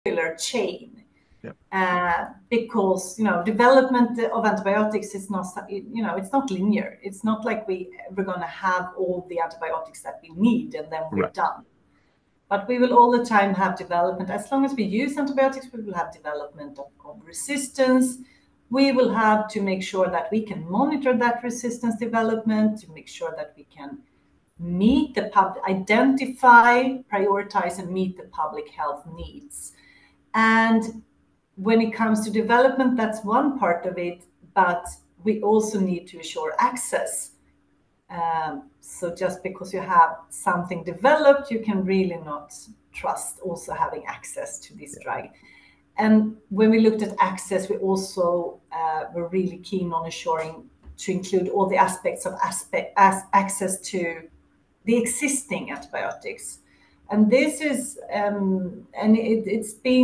Dr. Malin Grape, PhD - Ambassador on AntimicrobialResistance, Ministry of Health, Gov. Of Sweden
from @ProgressPotentialPossibilities

Dr. Malin Grape, Ph.D. - Ambassador on Antimicrobial Resistance, Ministry of Health and Social Affairs, Sweden, discussing her work in strengthening global efforts against AMR